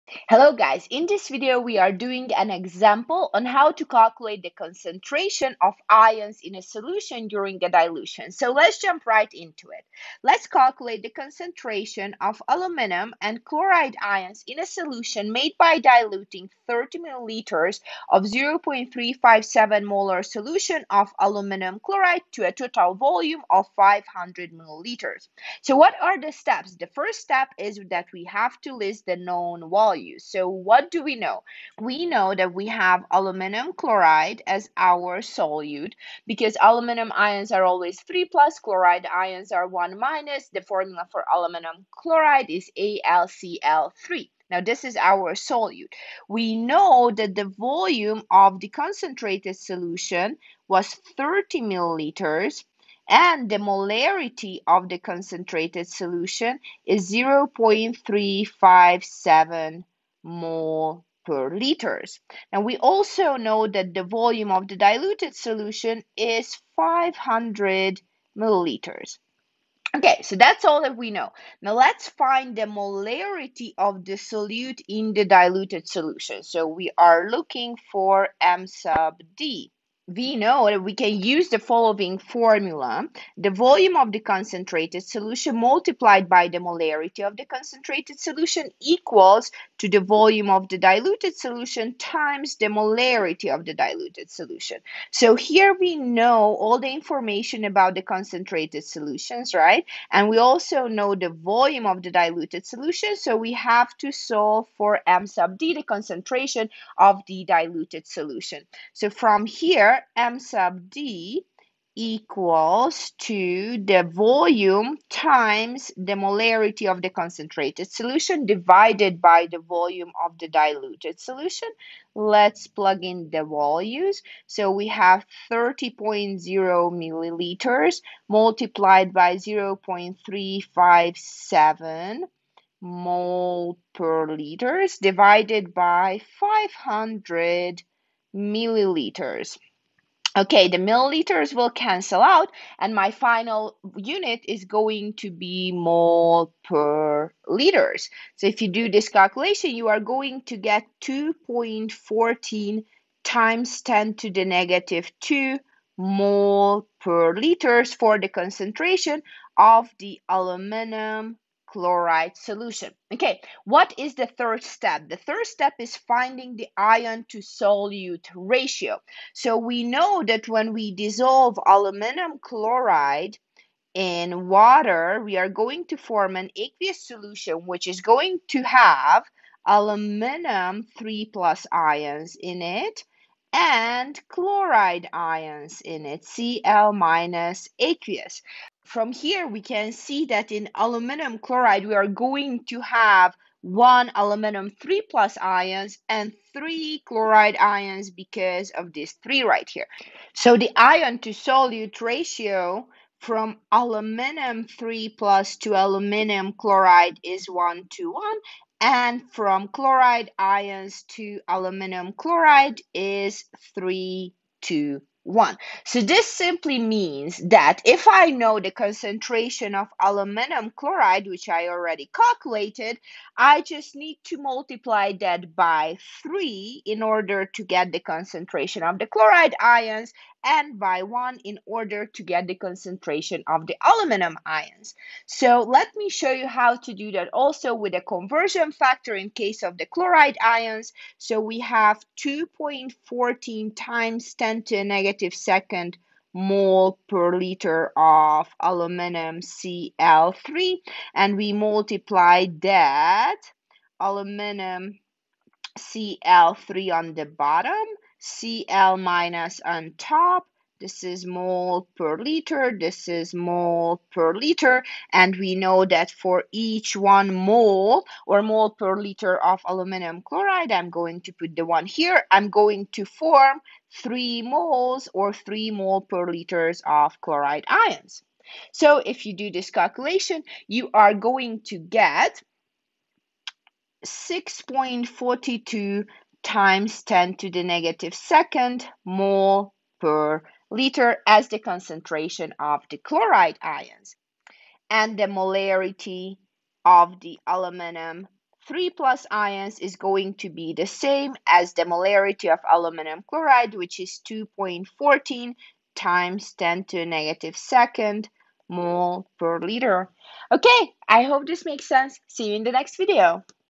Dilution example: how to calculate the concentration of ions in a diluted solution?
Let's do an example problem on the dilution of a solution.